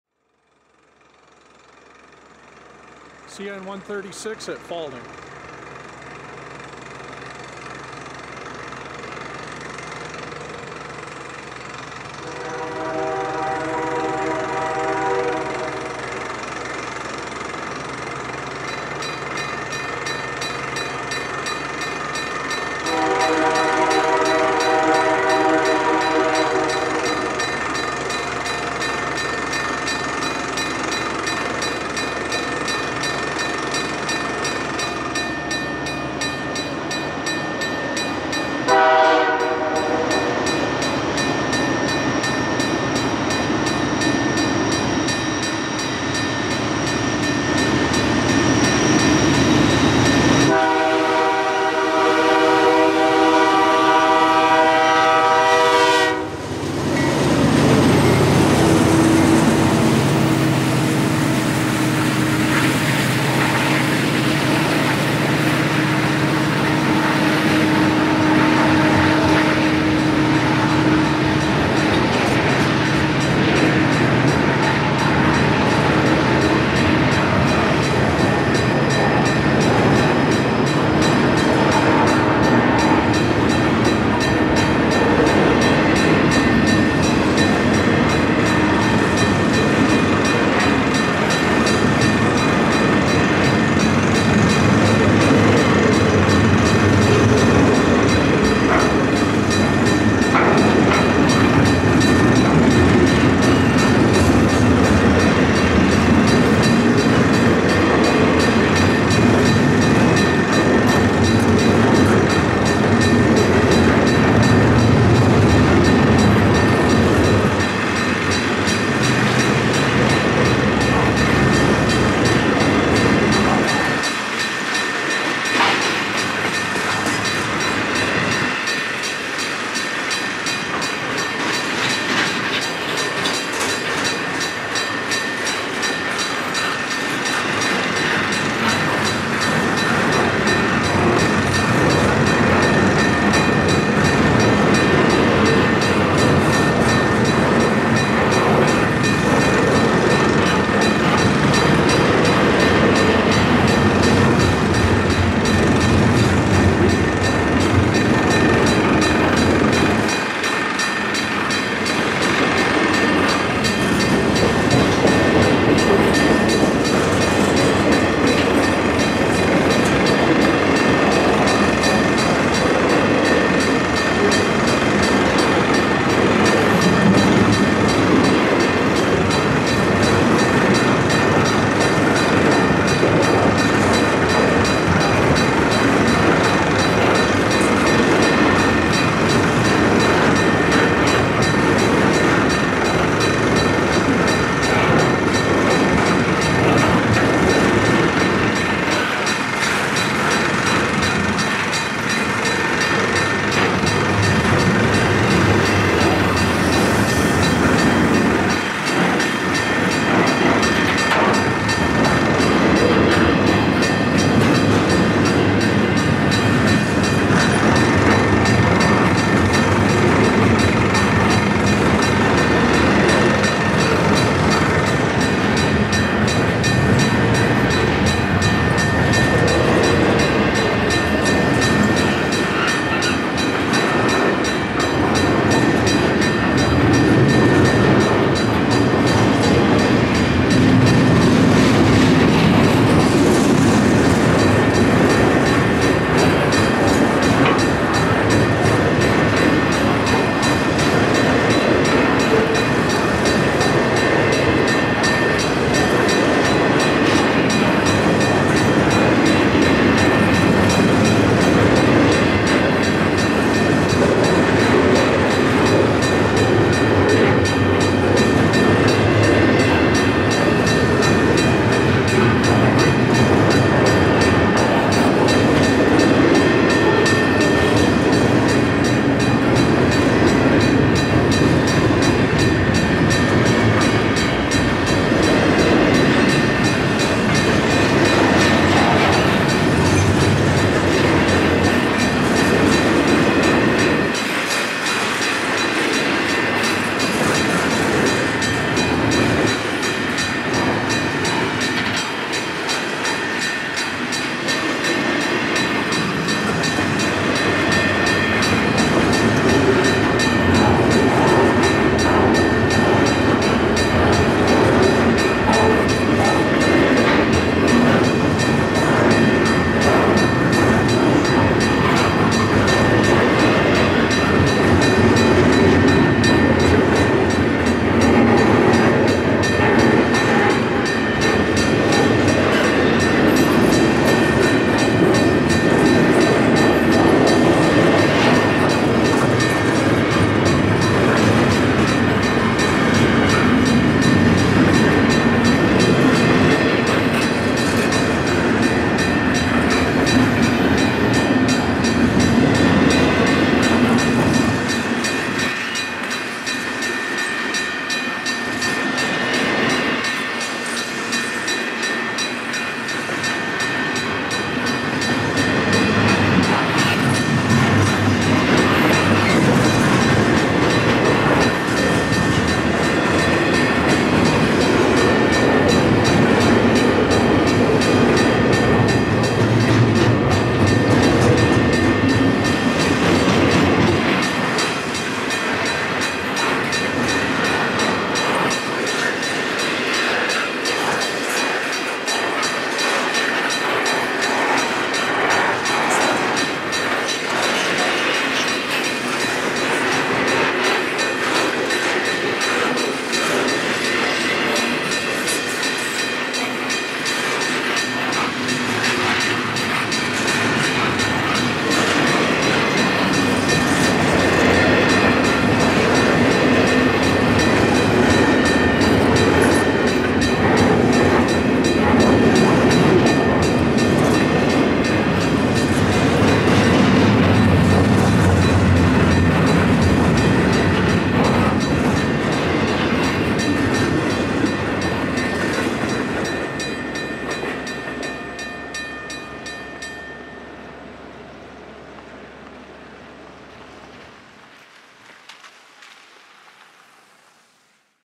REPRIEVE! CN 2543 at Falding (11JAN2018)
Canadian National 2543, CN 5720 (Train 360 Reroute - Winnipeg to Chicago) crossing Rankin Lake Road and heading for a meet with CN 105 at Woodward. The bell lives to ring another day!